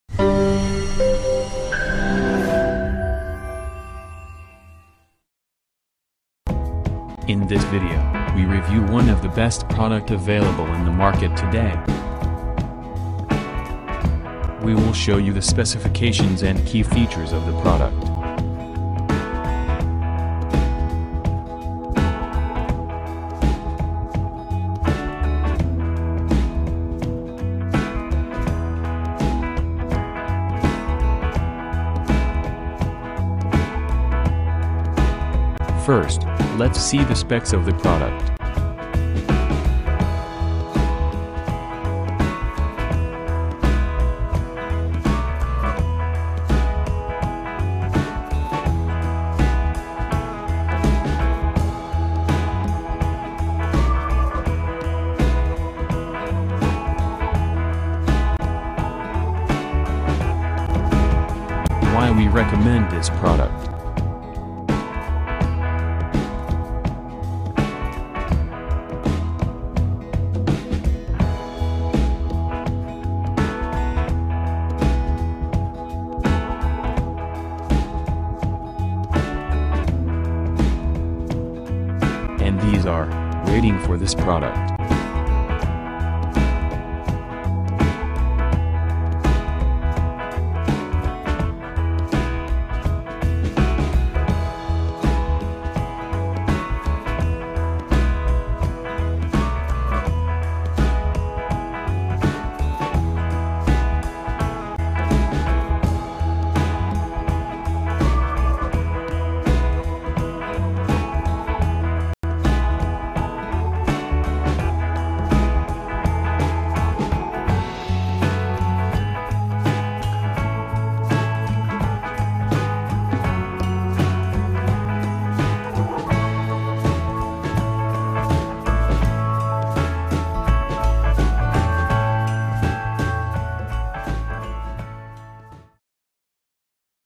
Hi-Tech Diamond 6" Lapidary Trim Saw Review - Best Table Saw For The Money
In this video we review Hi-Tech Diamond 6" Lapidary Trim Saw, one of the best table saw for the money. We will show you the specifications and key features of the product.

  (product price, dimensions, shipping, etc):

► Contents
0:00 Intro
0:34 Product Specifications
1:03 Product Review

► Why we recommend it
The perfect trim saw for trimming-out cabochon preforms in rocks, minerals and glass, or to cleanly trim facet material.
Includes vise attachment, TWO 6" diamond saw blades, spray shield, Allen wrench and complete written and visual instructions.
Powerful 1/4 hp, heavy-duty, ball bearing motor with variable speed, delivering speeds from 800rpm to 3,400rpm.
Made of lightweight, rustproof, impact-resistant material with small footprint.
Manufactured in the U.S.A. Comes backed with a one-year warranty.